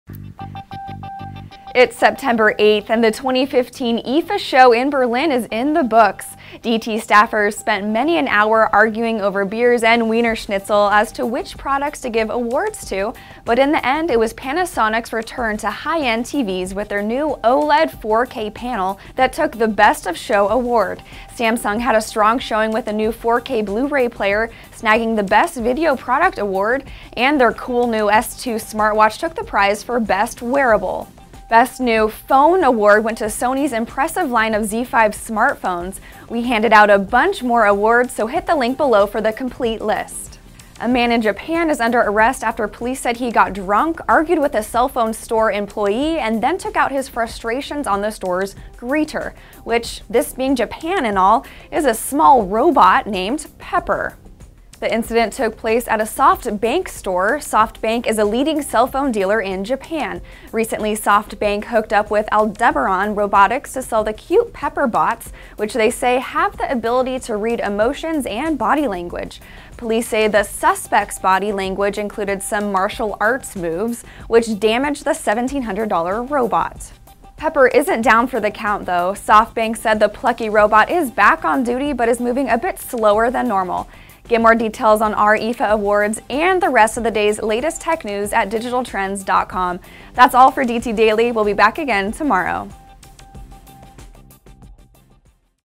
Drunk guy attacks robot greeter in Japan: DT Daily
Our top tech picks from the IFA 2015 show in Berlin; man takes his frustrations out on a SoftBank Pepper robot in Japan, gets arrested.